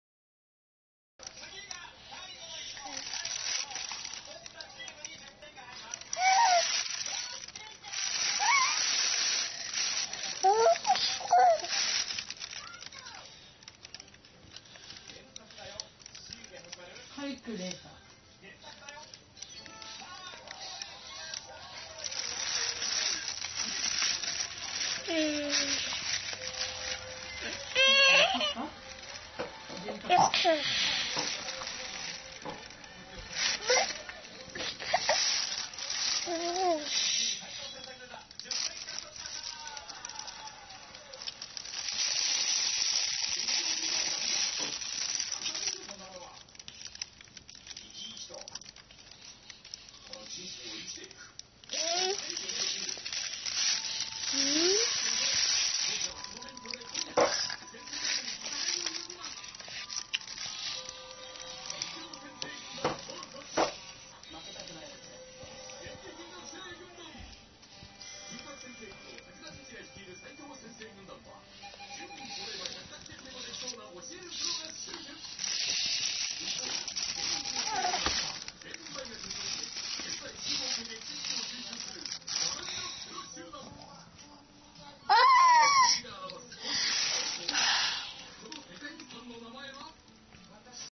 柿ドロボー
実家でもらった柿を台所に置いておいたら、侵入者に一個盗まれました。
食い意地はってるなあ、誰に似たかなあ